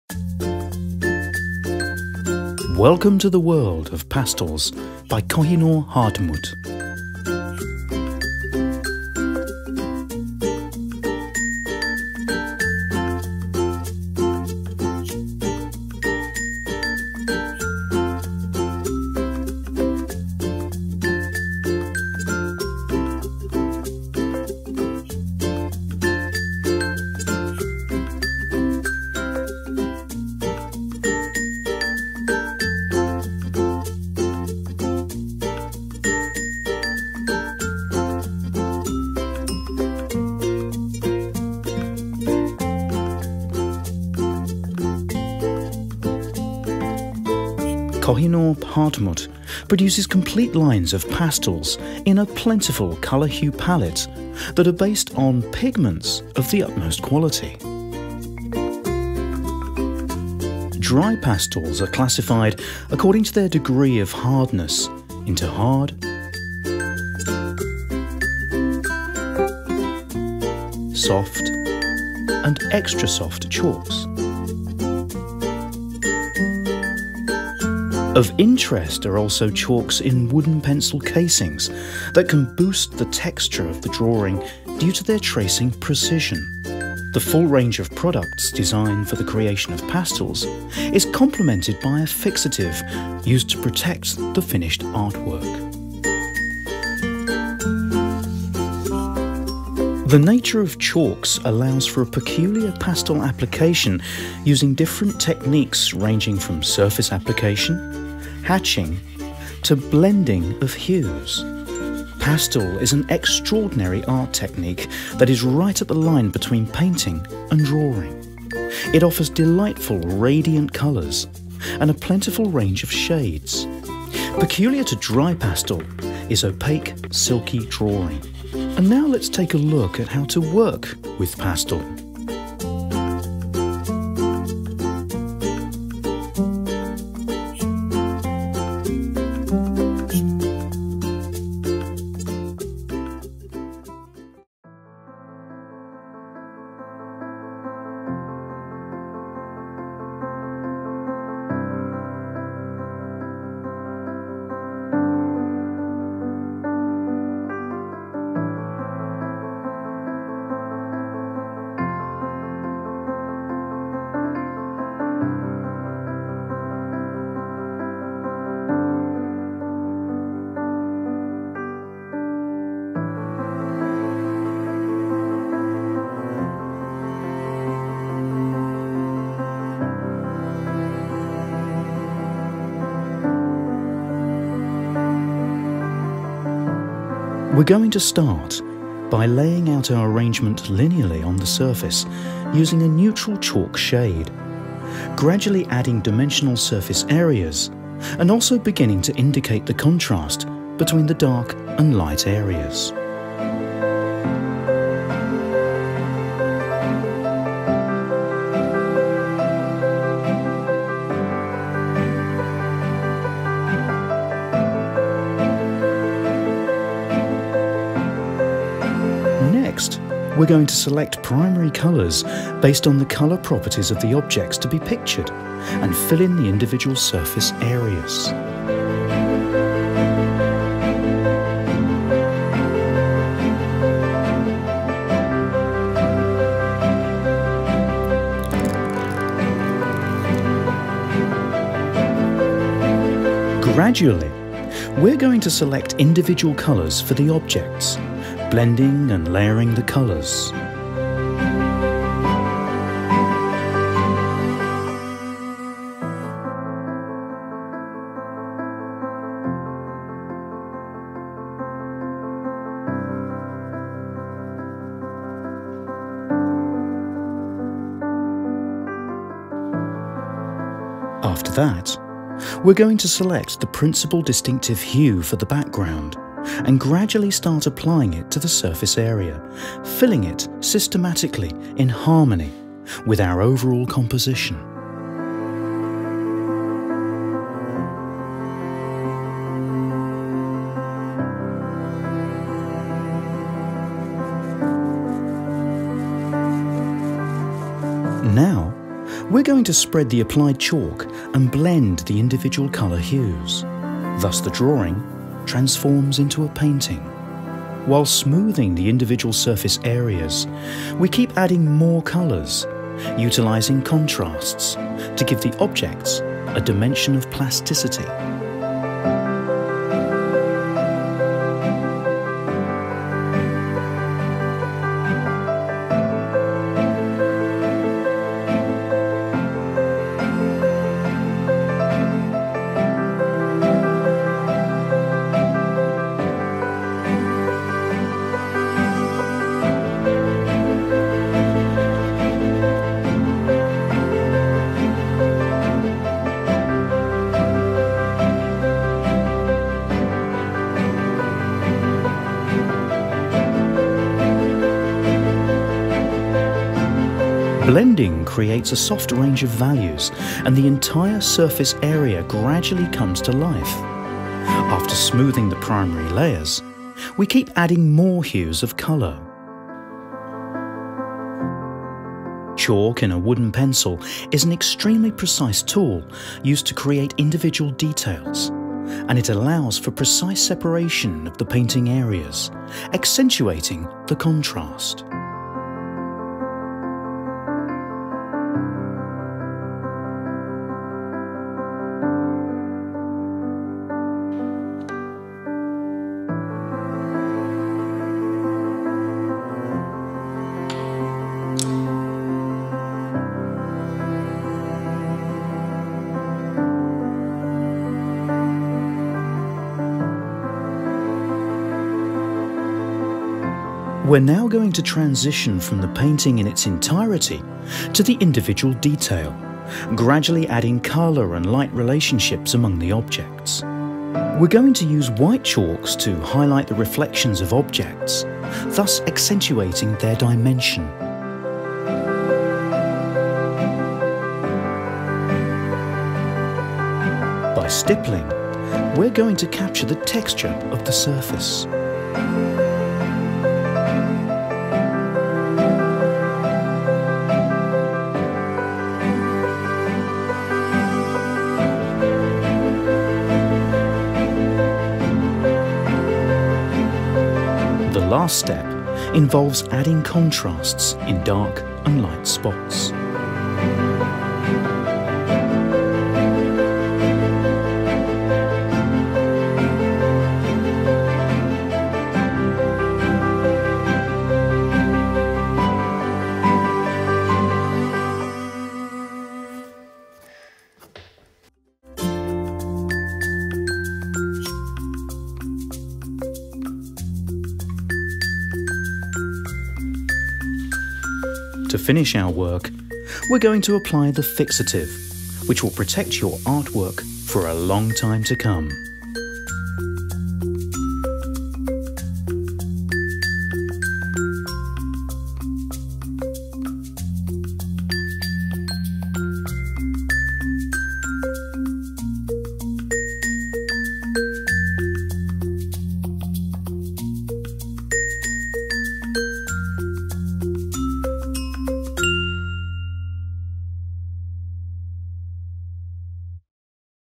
KOH-I-NOOR HARDTMUTH - How to use PASTELS (ENG)
Do you want to learn everything about pastels? Go ahead and watch this video - The World of Pastels.